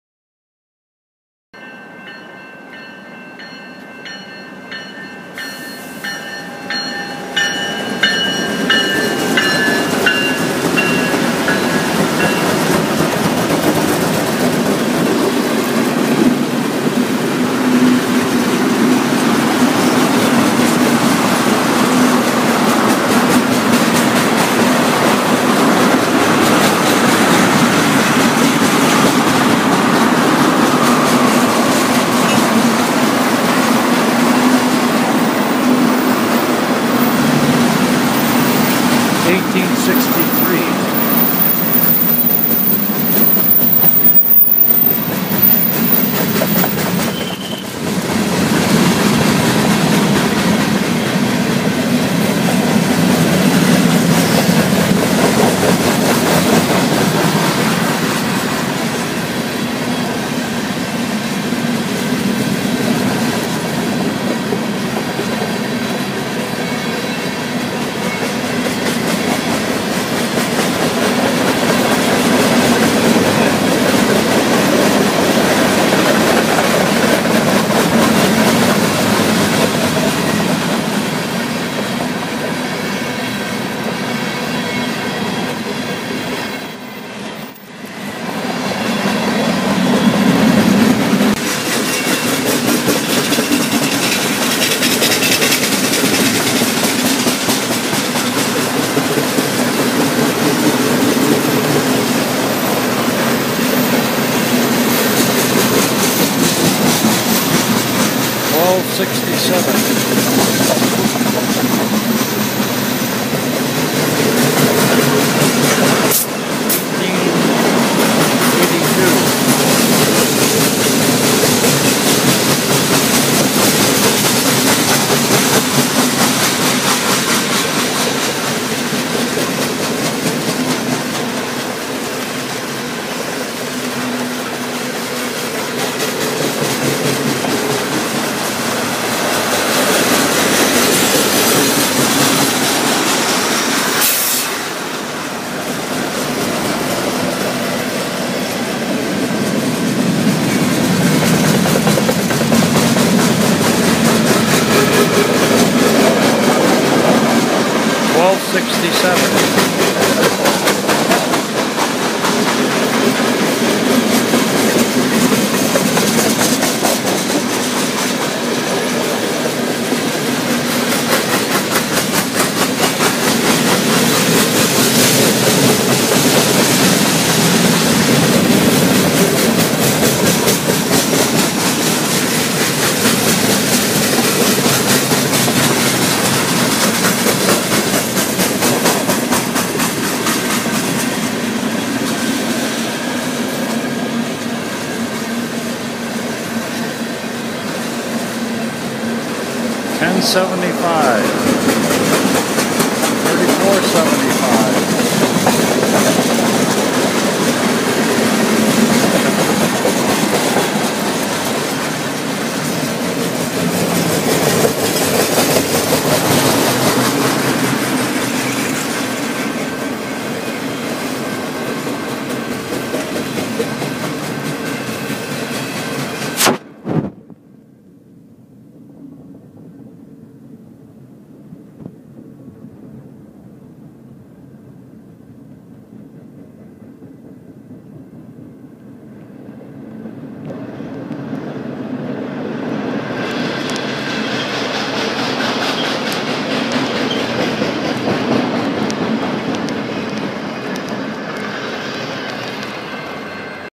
WSMRailVideos.Early Morning Freight.
Caught CN near the Ii'Le Perrot crossing, After tapping CN Passing over CN Bridge 1/4 away.